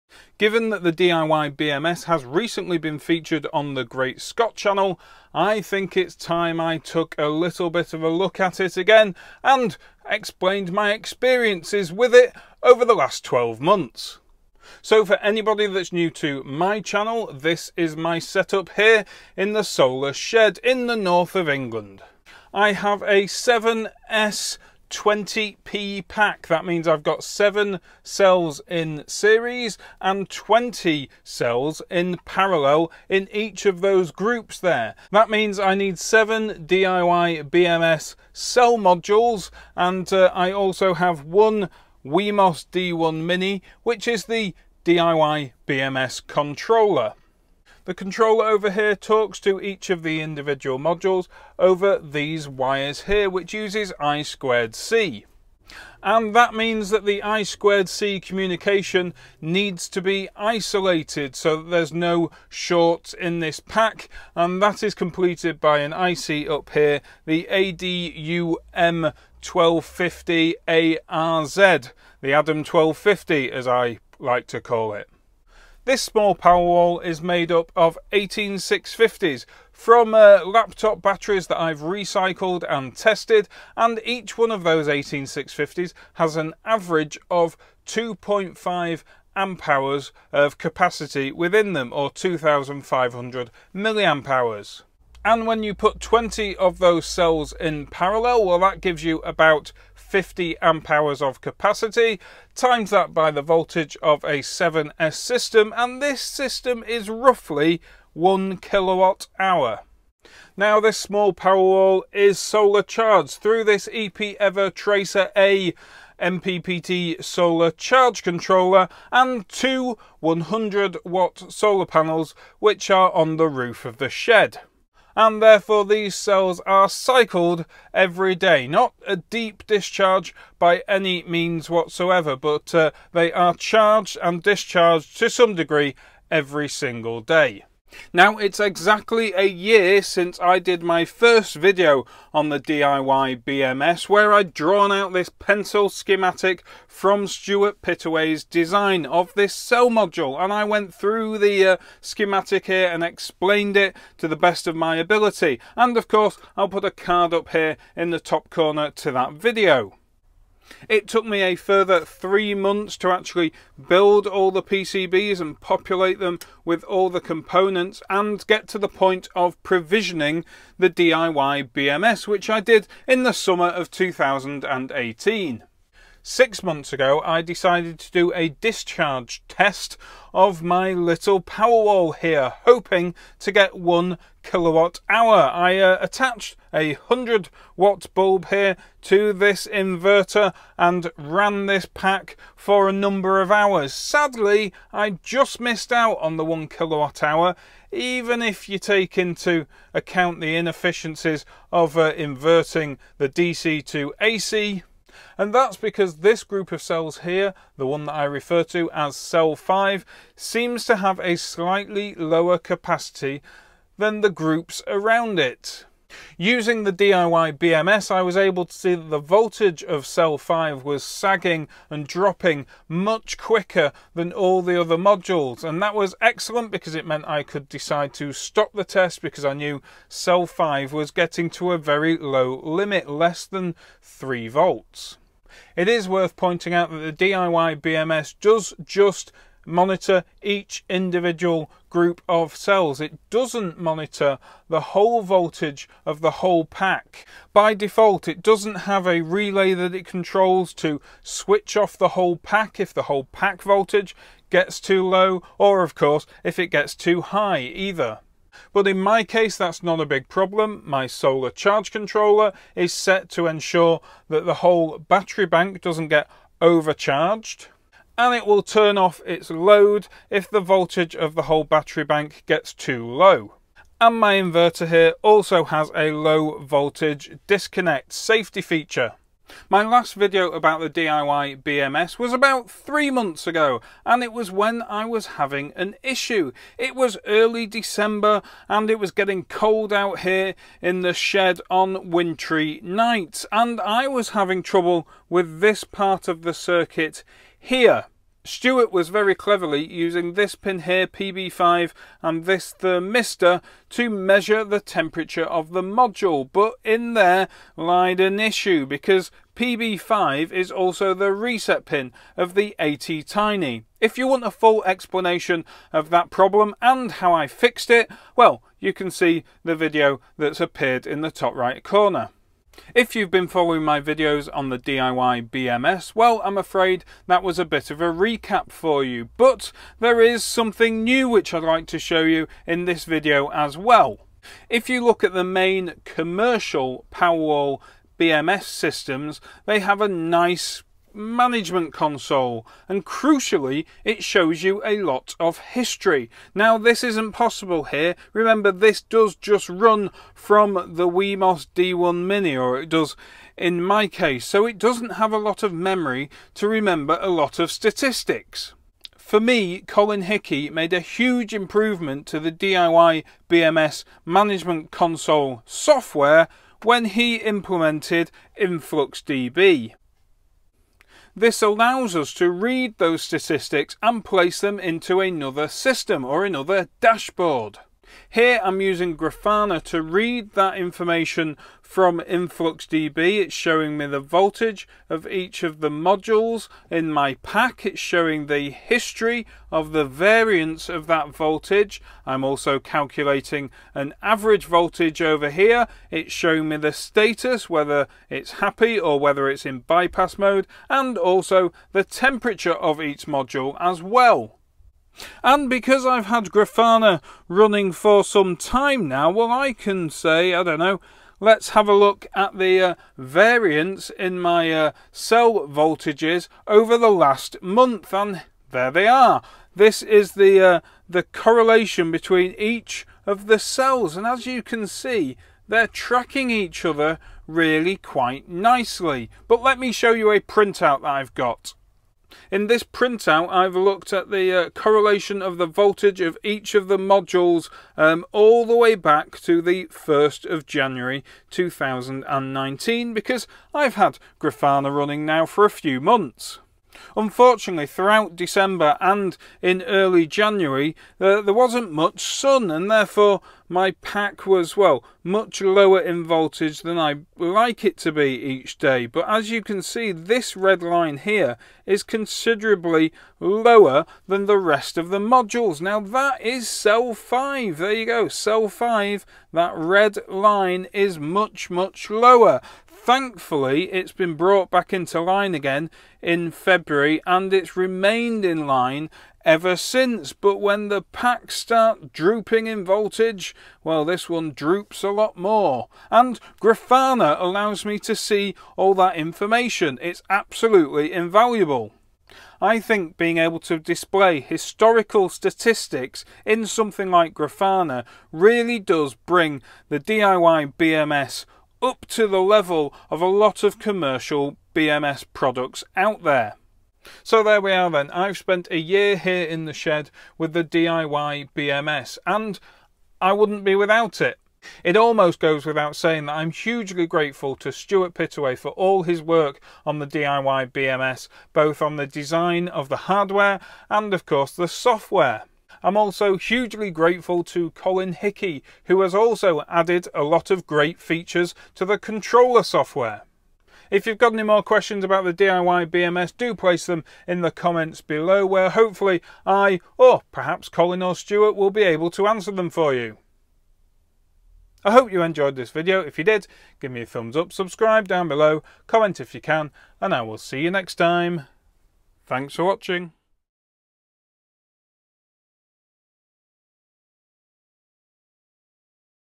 A year with the diyBMS
I started to build my diyBMS a year ago.  It has been an interesting journey and in this video I review both the ups and the downs.

Items used in this video:
Tracer A MPPT Solar Charge Controller
EPEver STI inverter
Wemos D1 Mini (Clone)